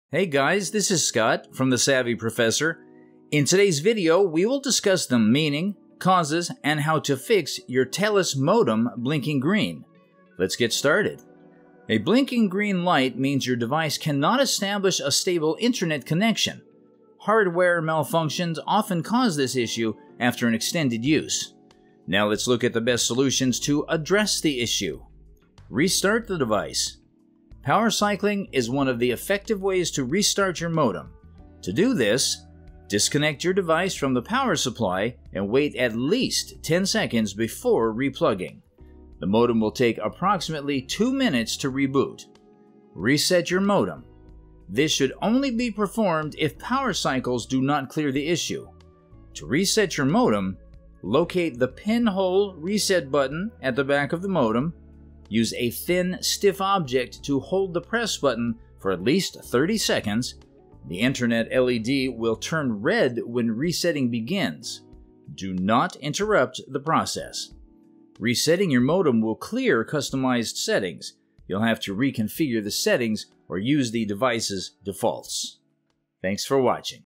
How To Fix Telus Modem Blinking Green (Hardware Malfunctions - Step-By-Step Guide To Solve It!)
How To Fix Telus Modem Blinking Green (Hardware Malfunctions - Step-By-Step Guide To Solve It!). In this video tutorial I will show you how to fix Telus Modem blinking green.

A blinking green light means your device cannot establish a stable internet connection. Hardware malfunctions often cause this issue after an extended use.

Now, let’s look at the best solutions to address the issue.
Power cycling is one of the effective ways to restart your modem. To do this:
• Disconnect your device from the power supply and wait at least ten seconds before re-plugging.
• The modem will take approximately two minutes to reboot.

Reset your modem
This should only be performed if power cycles do not clear the issue. To reset your modem:
• Locate the pin-hole reset button at the back of the modem.
• Use a thin, stiff object to hold-press the button for at least thirty seconds. The internet LED will turn red when resetting begins.
• Do not interrupt the process.
Resetting your modem will clear customized settings. You’ll have to reconfigure the settings or use the device’s defaults.